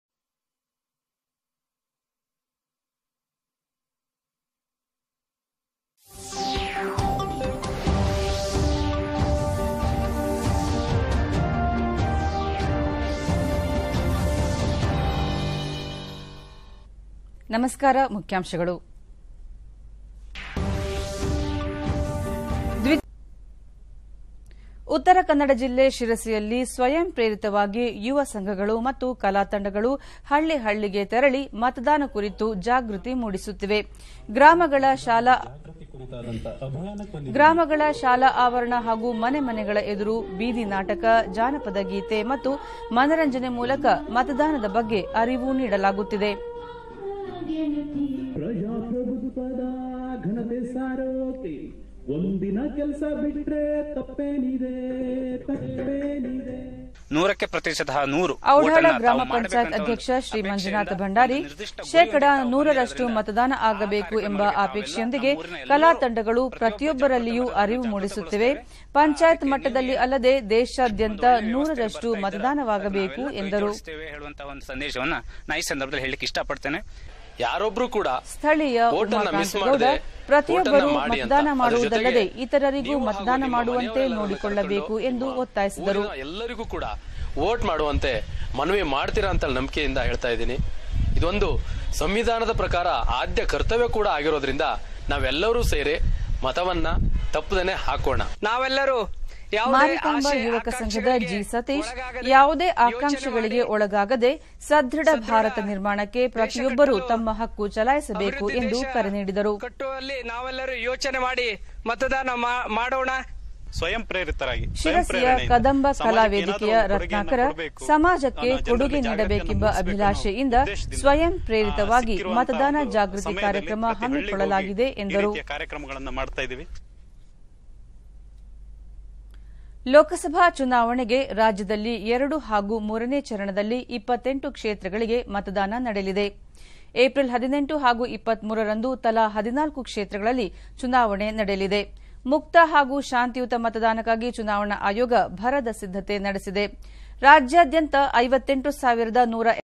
*ಮತ-ಹಿತ* ಮತದಾನ ಜಾಗೃತಿ ಅಭಿಯಾನ
🙏52ಕ್ಕೂ ಅಧಿಕ ದೇಶಗಳಲ್ಲಿ ಬಿತ್ತರಗೊಂಡ ಶಿರಸಿ ರತ್ನಾಕರ  ನಿರ್ದೇಶನದ ಕದಂಬ ಕಲಾವೇದಿಕೆ ಹಾಗೂ ಶಿರಸಿ ಕರೋಕೆ ಸಿಂಗಿಂಗ್ ಸ್ಟುಡಿಯೊ ಕಲಾವಿದರು ಅಭಿನಯಿಸಿದ ಅಭಿಯಾನ ಎಲ್ಲರೂ ವೀಕ್ಷಿಸಿ 🙏thanks to chandana tv and  oudala youngsters